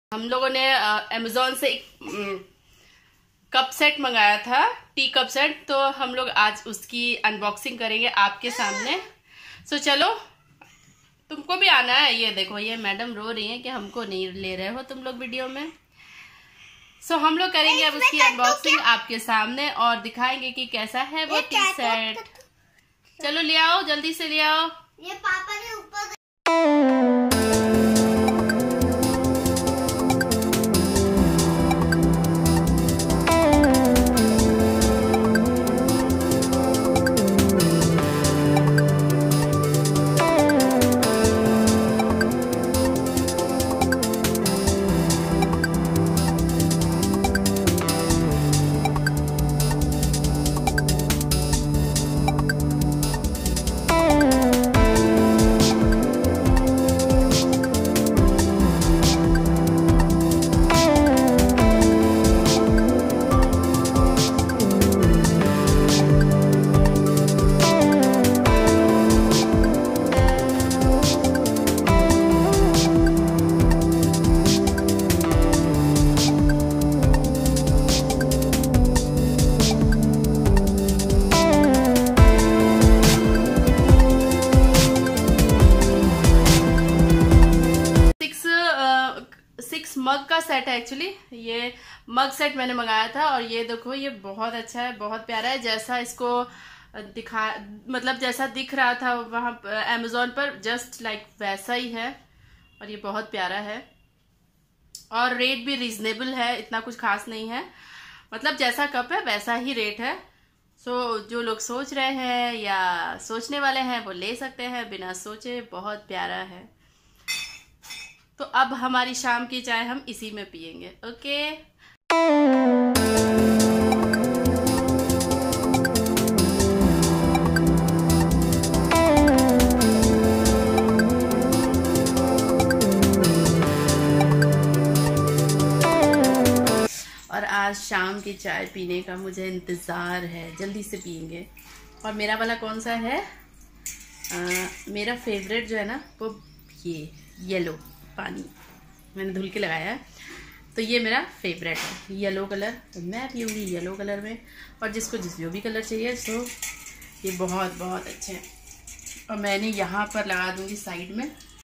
Coffee Mug -unboxing and review of coffee mug set | daily blogger Indian | कॉफ़ी मग अनबोक्सिंग
Coffee Mug: -
                          unboxing  and review of coffee mug set | daily blogger Indian |
                           कॉफ़ी मग अनबोक्सिंग और रिव्यु